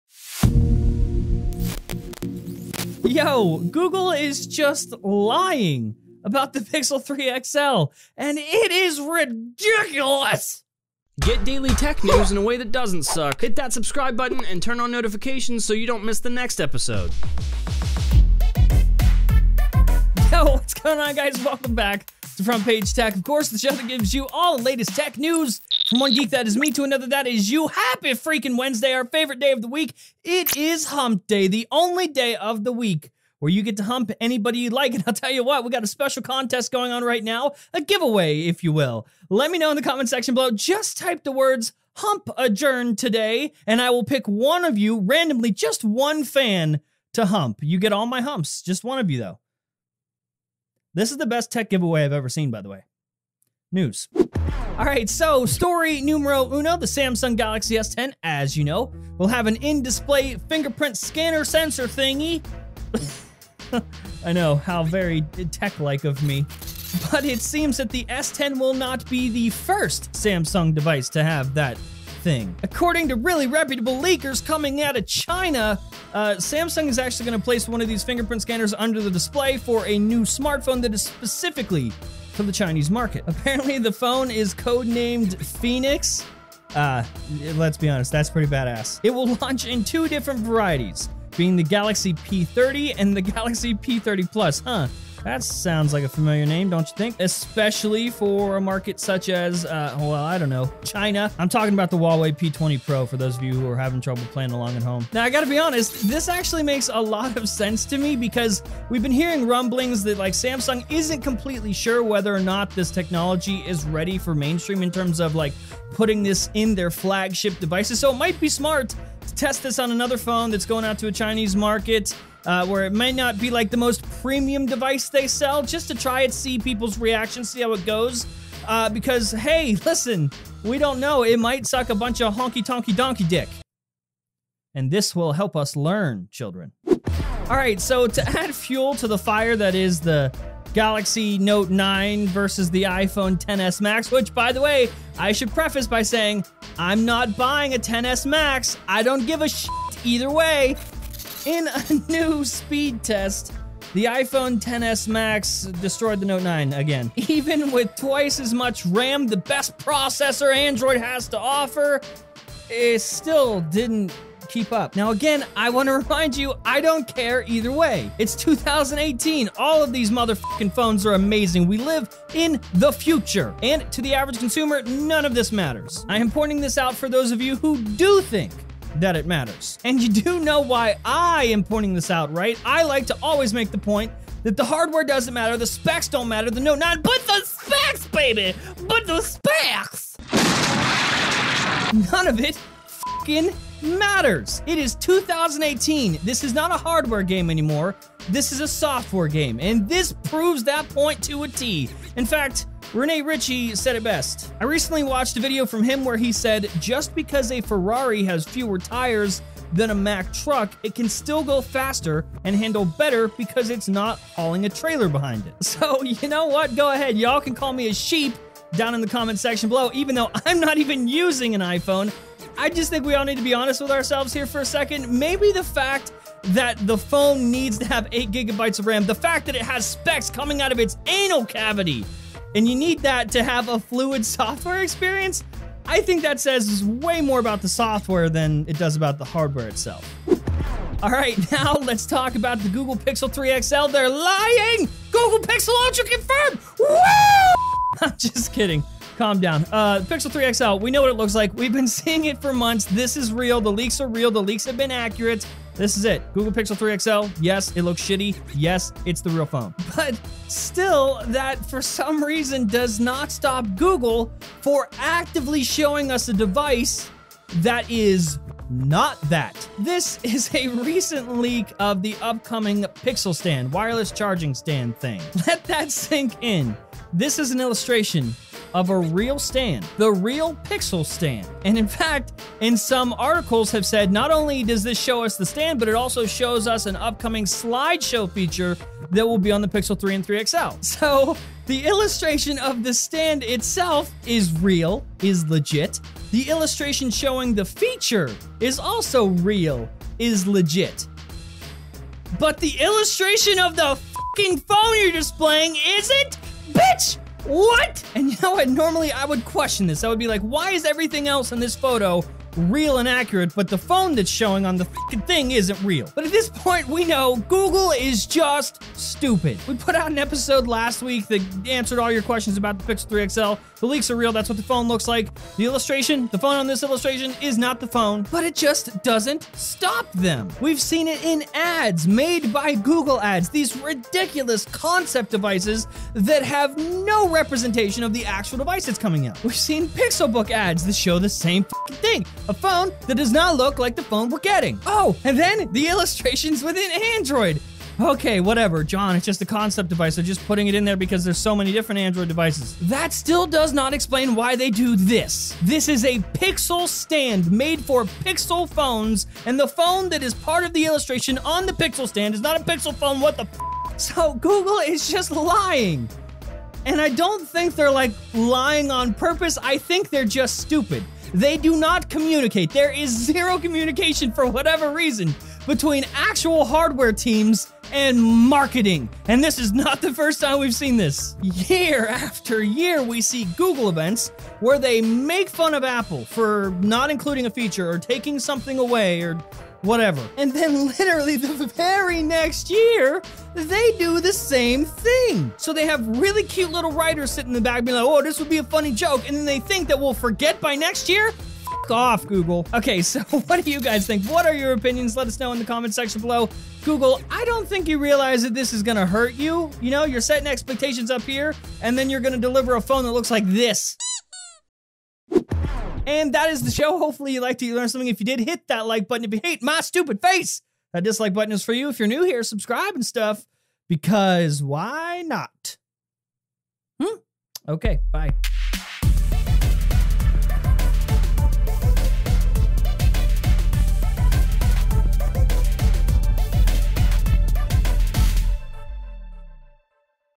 Google is LYING about the PIXEL 3 XL
EP. 740 - Google is LYING about the PIXEL 3 XL
There is ZERO communication between Google hardware and Google marketing and it's causing google to look ridiculous.